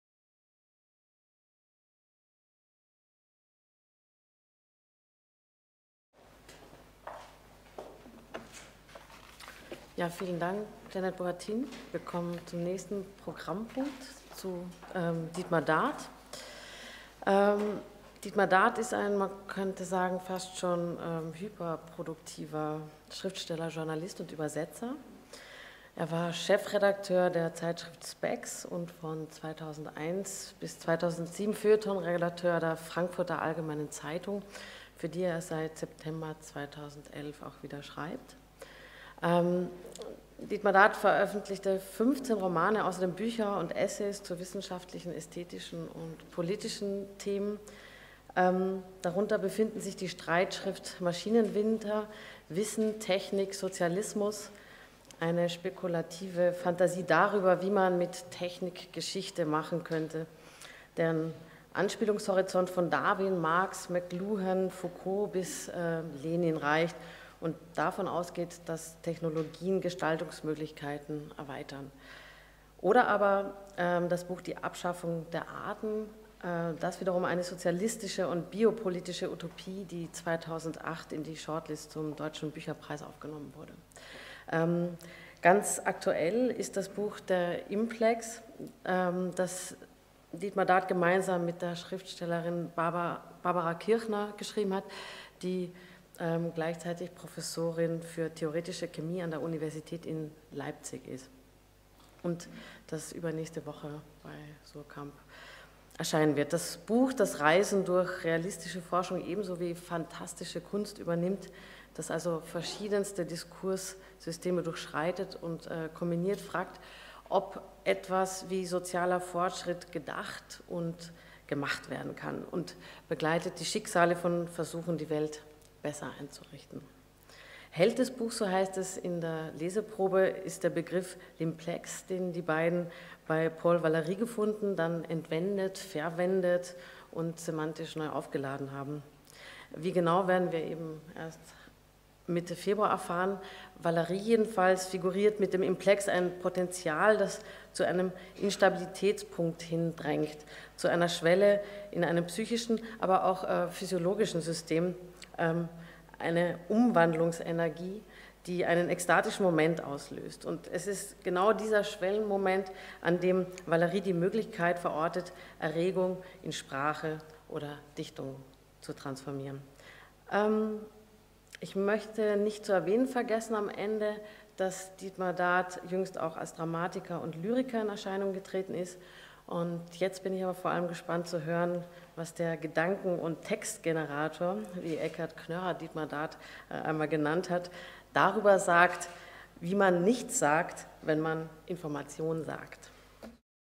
7a - Technik und Poetik - Jeannie Moser stellt Dietmar Dath vor
Technik und Poetik - Symposium in Erinnerung an Walter Höllerer